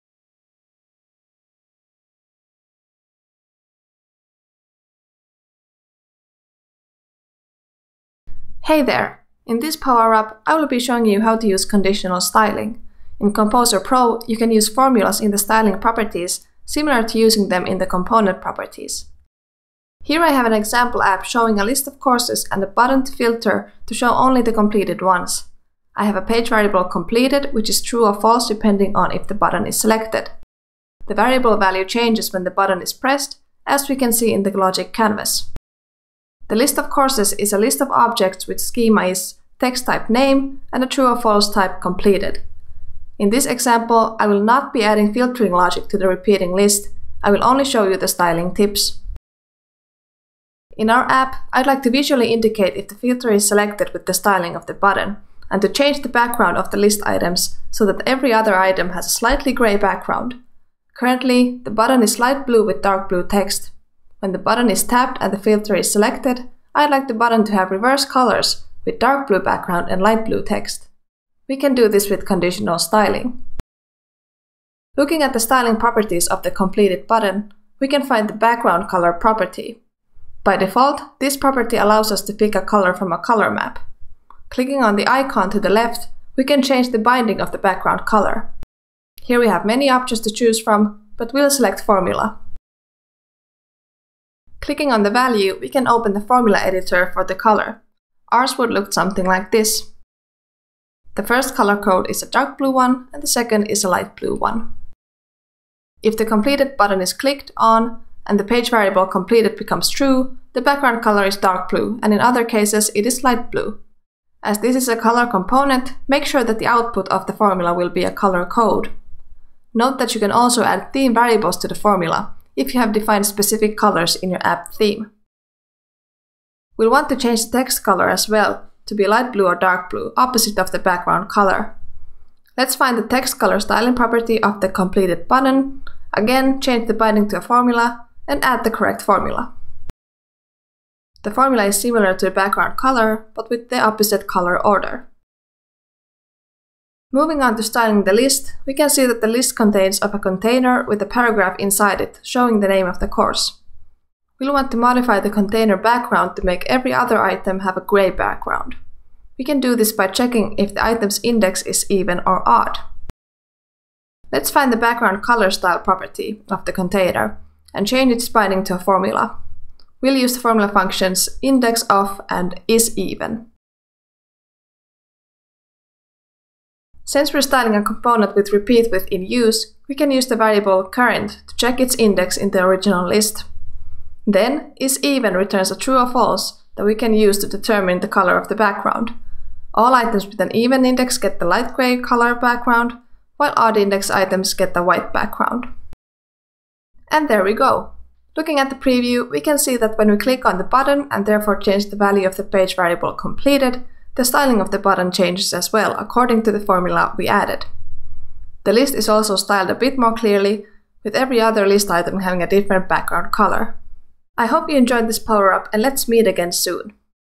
No-Code Power-up: Conditional Styling
In this video you'll learn how to do conditional styling in AppGyver Composer Pro.